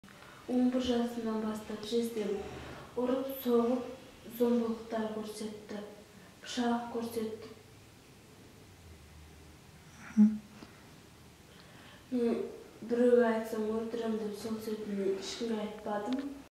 Бала туған оқушы қыз
Зорлық-зомбылық кесірінен ерте жүкті болып, кәмелетке толмай босанған оқушы қыз баласының әкесі жездесі екенін айтады. Шымкент, 10 қараша 2016 жыл.